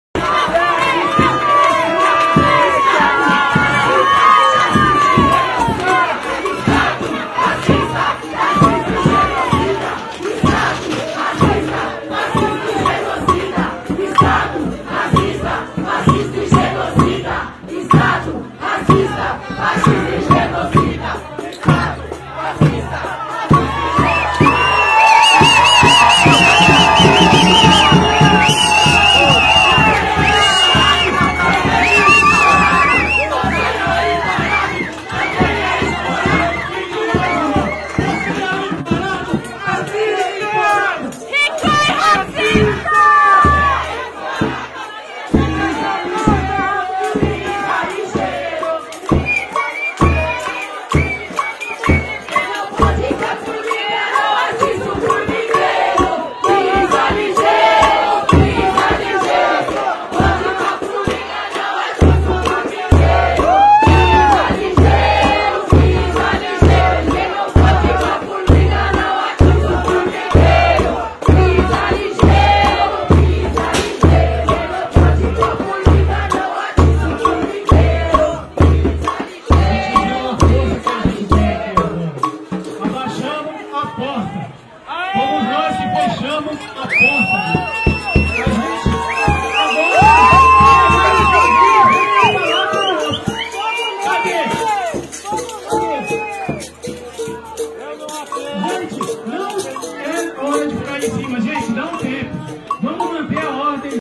MANIFESTANTES FECHAM PORTA DE SUPERMERCADO RICOY, NA ZONA SUL DE SP
Manifestantes antirracistas fecham o supermercado Ricoy, na Zona Sul de São Paulo, contra a tortura de um adolescente de 17 anos.